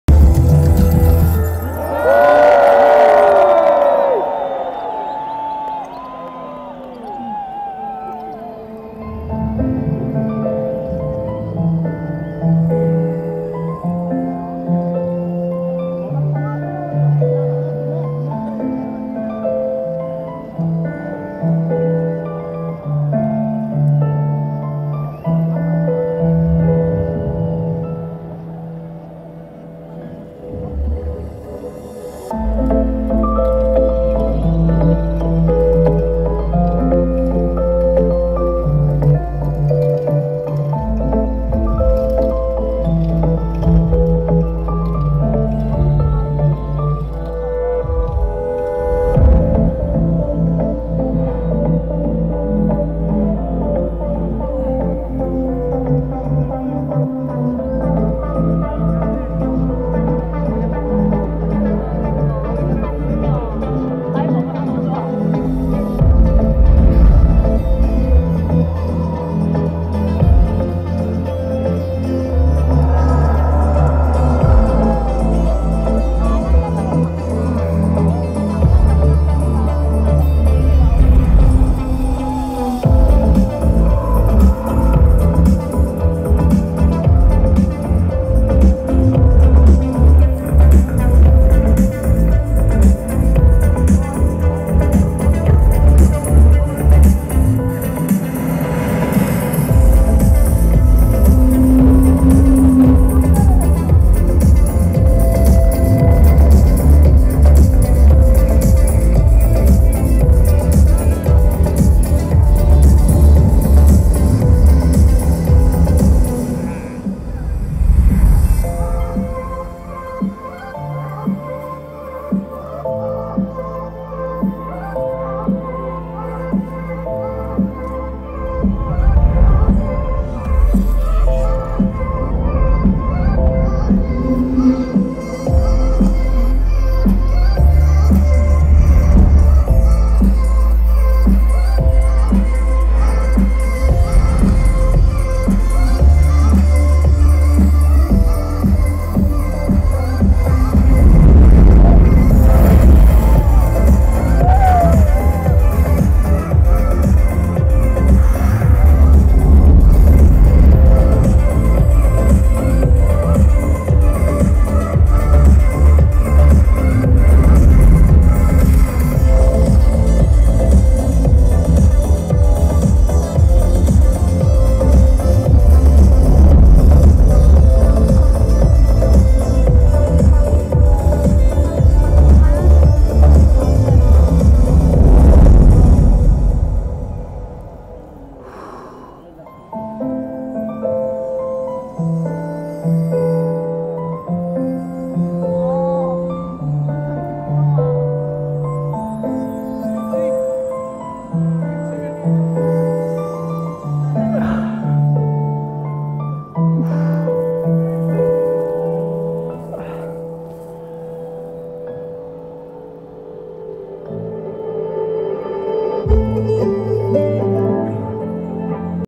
2018 12 31 Nouvel an à Paris 007
ANNEE : 2018
MOIS : 12
JOUR : 31

LIEU : PARIS 8ème
Avenue des Champs Élysées

EVENEMENT : Nouvel an
Sons et lumières sur l'Arc de Triomphe

PERSONNAGE(S) :